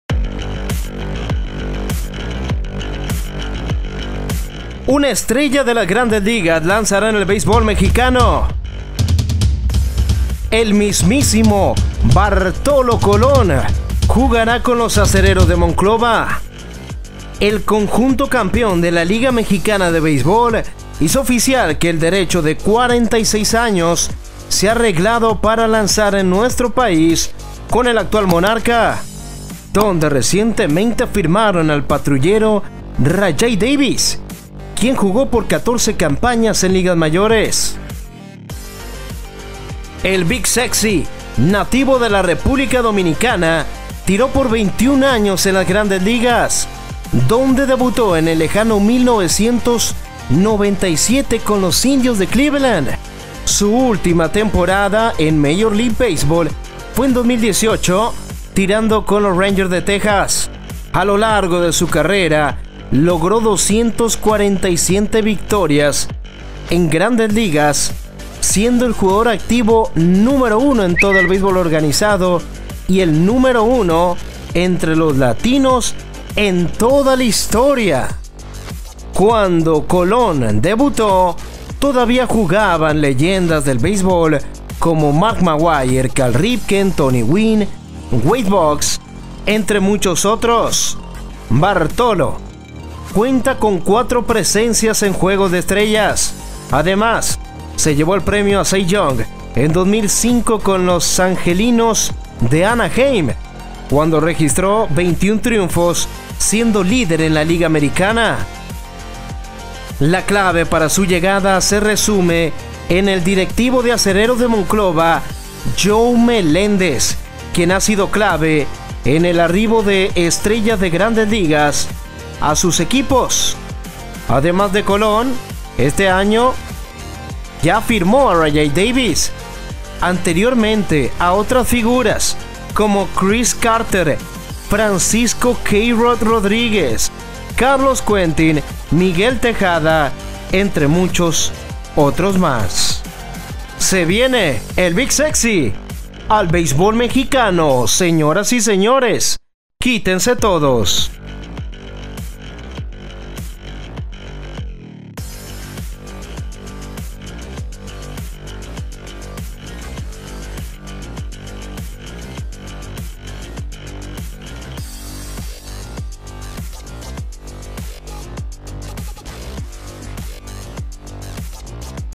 La leyenda BARTOLO COLÓN decide jugar en México
El legendario lanzador dominicano, pitcher latino con más triunfos en todas las Grandes Ligas, ha firmado para jugar en el beisbol mexicano a sus 46 años. El 'Big Sexy' busca seguir brillando en el montículo.